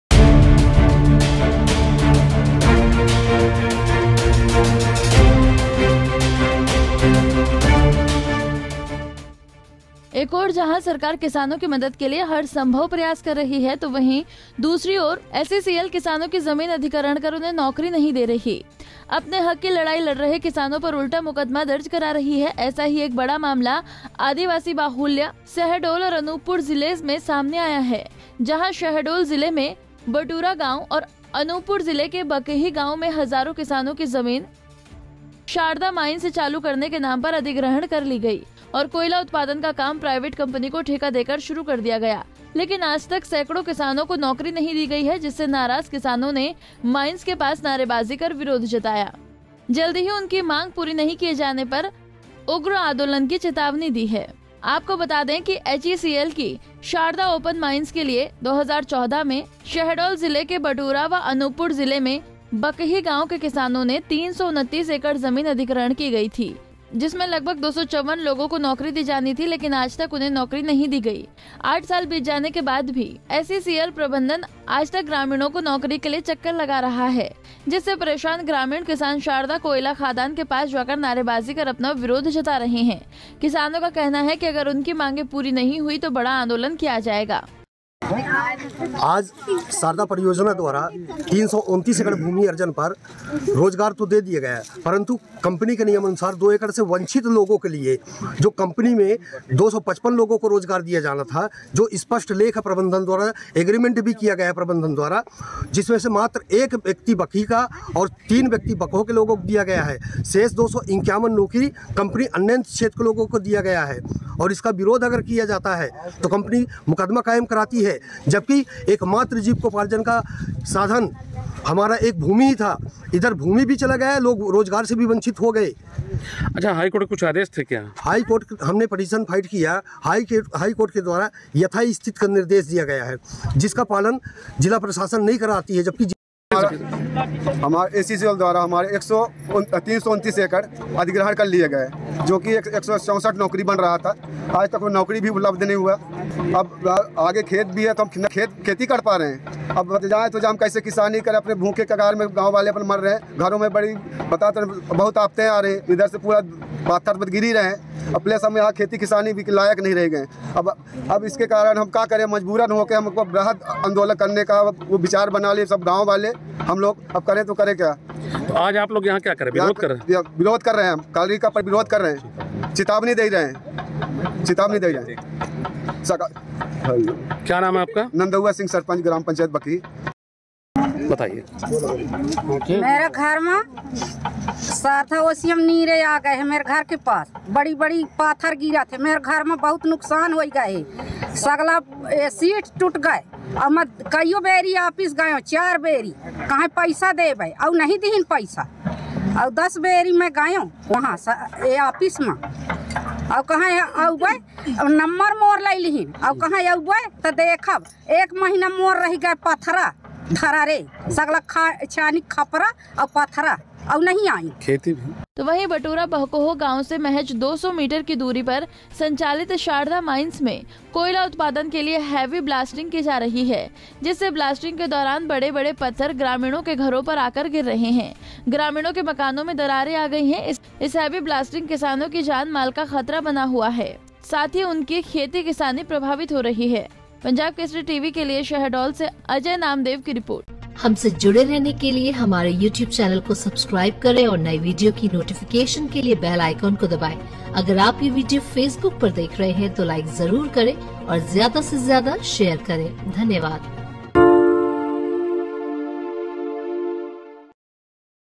SECL ने हड़प ली किसानों की जमीन, नौकरी के नाम पर दर्ज करा दिया केस! किसानों ने दी आंदोलन की चेतावनी
SECL ने किसानों की हड़प ली जमीन
नौकरी मांगने पर करा रहे मुकदमा
नाराज ग्रामीणों के माइंस के सामने जताया विरोध